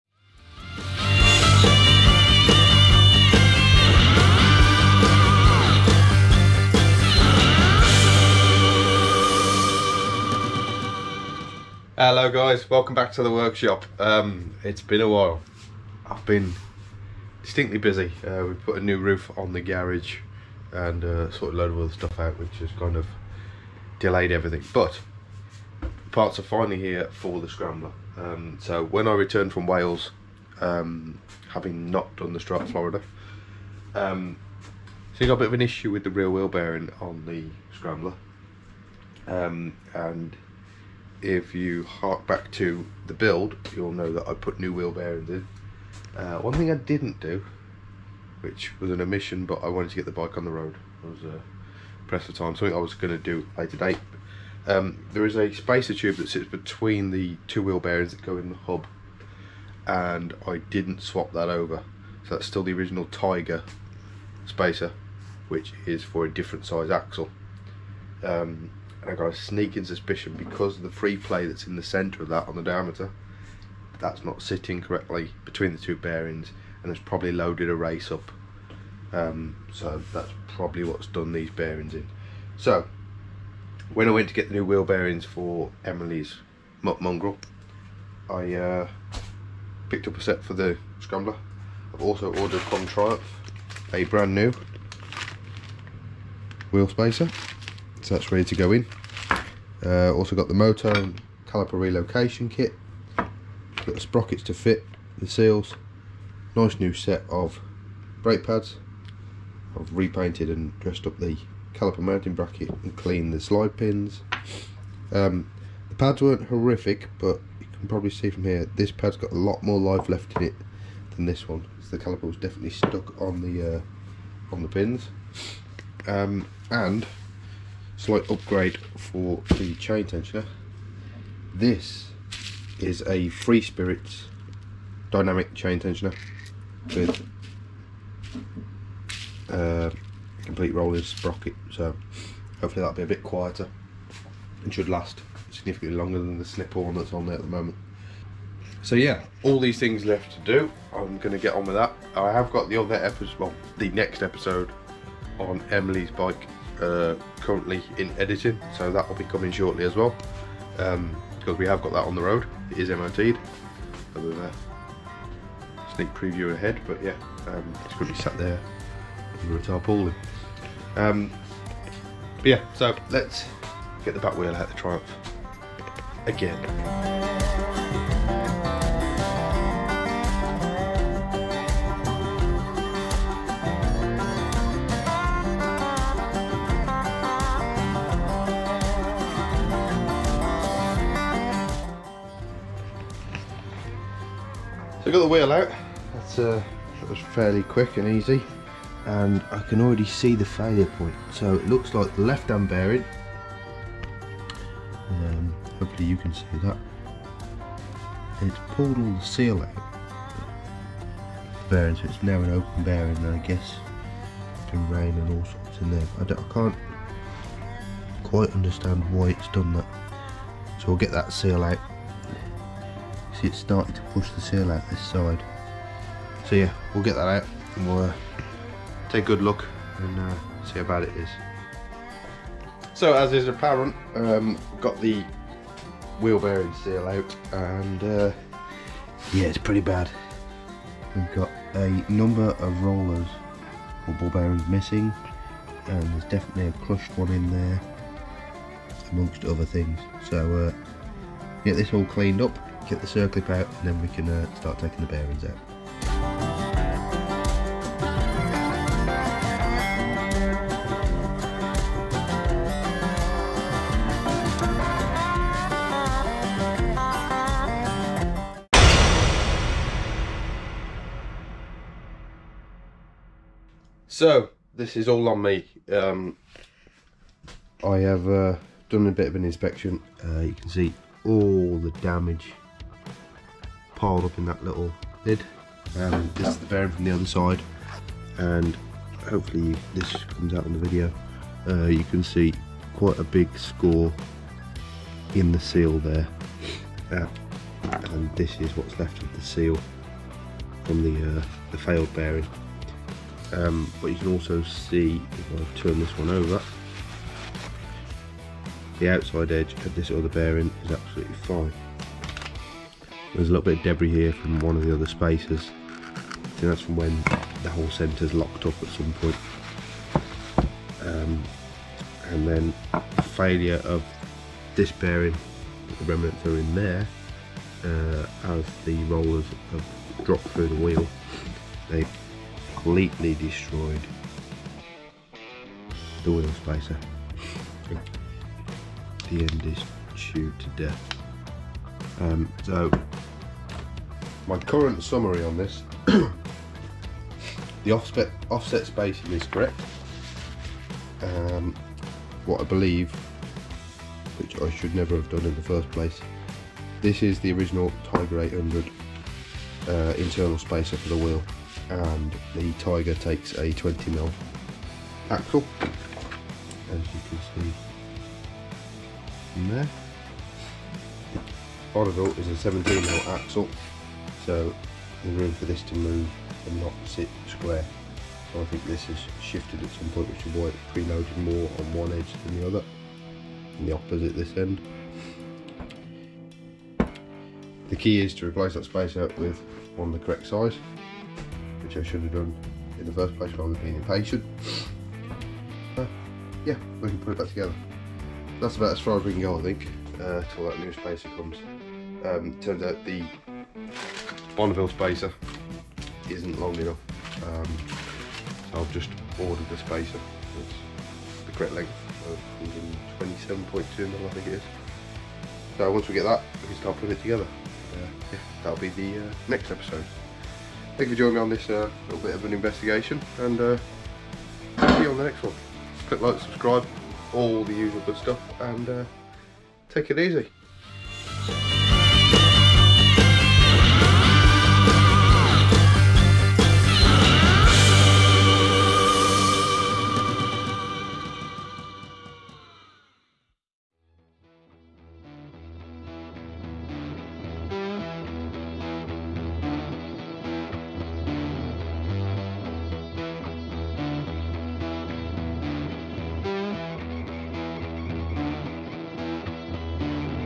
I’ve BROKEN IT… Again! | PROJECT TRIUMPH BONNEVILLE
Investigating a failed rear wheel bearing on my Bonneville Scrambler build and it turns out, maybe, it was all my own doing!

Fitting a Tiger 800 rear wheel to a Bonnie.

Join me for the latest update on the Triumph project where I begin the fun task of installing the modified wiring harness, unbox the new back wheel and a few other things.

Original bike: 2010 Triumph Bonneville SE

❱Setup So Far:
GoPro Hero 8 Black - Bought Used
GoPro Case (Artman)
GoPro Arms (HYCKee)